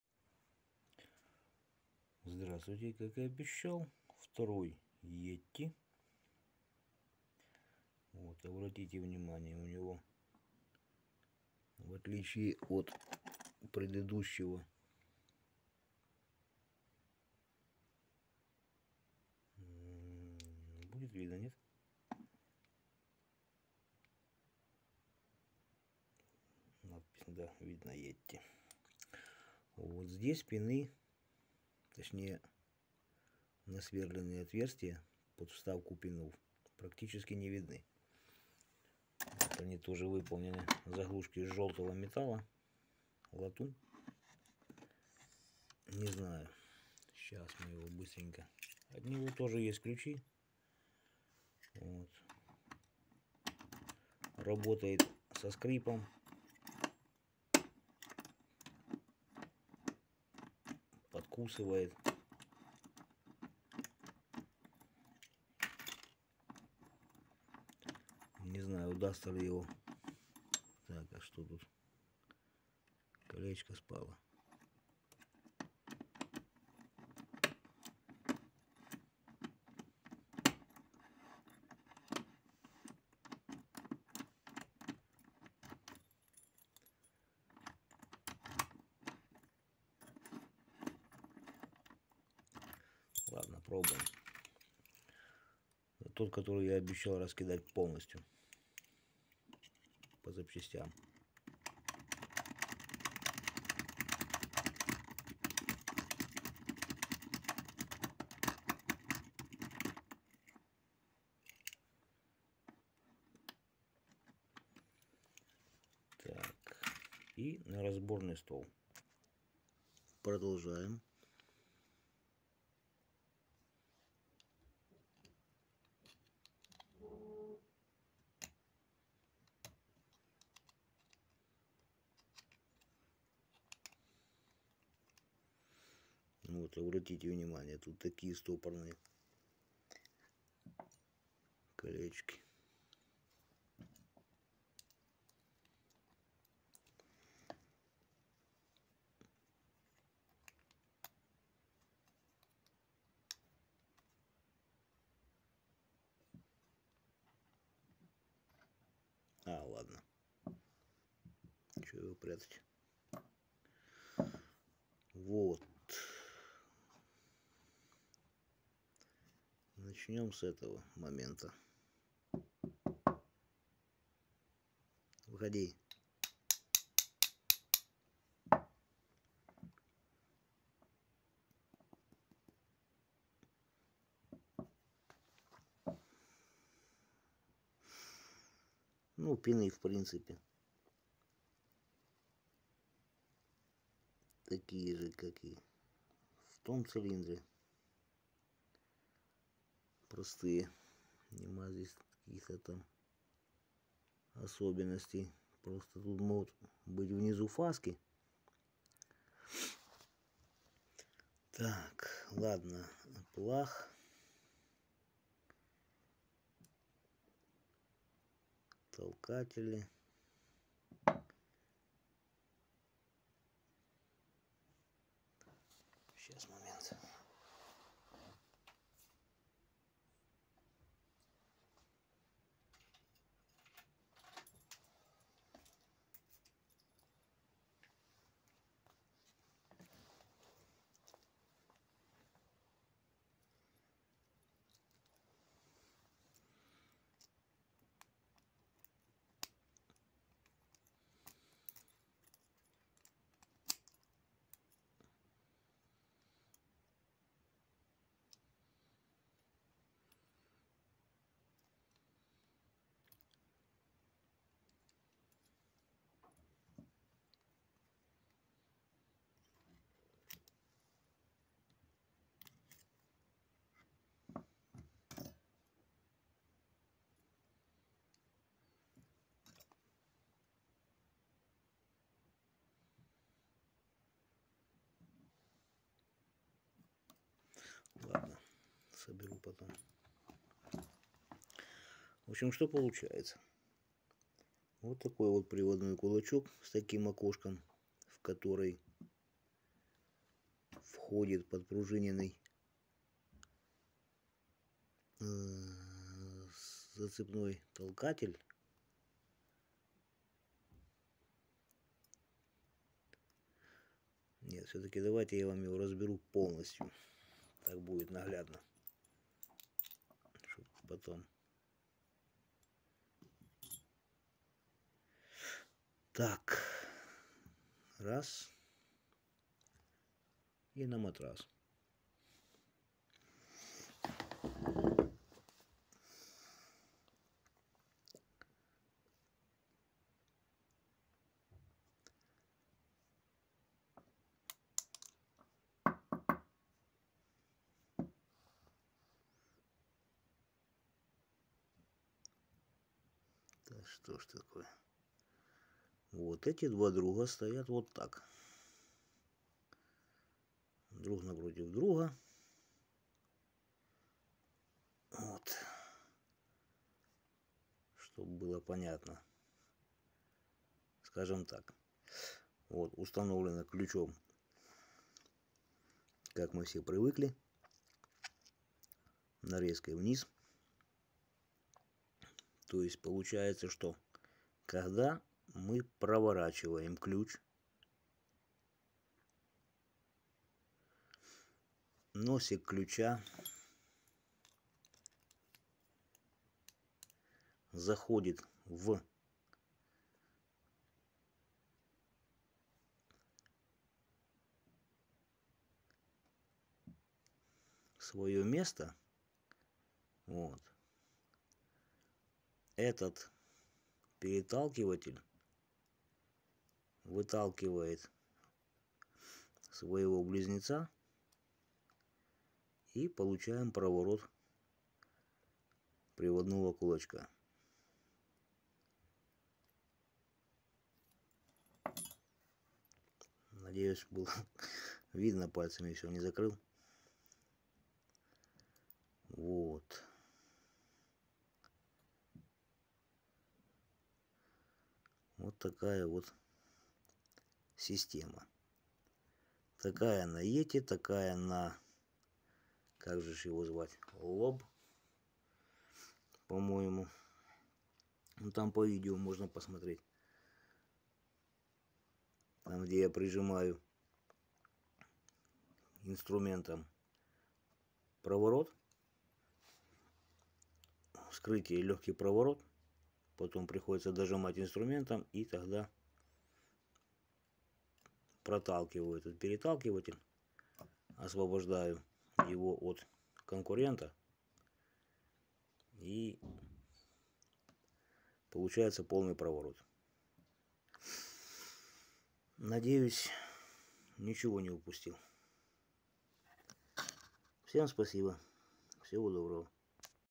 Вскрытие замка yeti с разборкой цилиндра
В этом видео у нас будет вскрытие ещё одного цилиндра yeti. Раннее мы уже показывали вскрытие замка этого производителя. Сколько по времени продержится этот замок и какие пины установлены в этом цилиндре, смотрите в этом видео.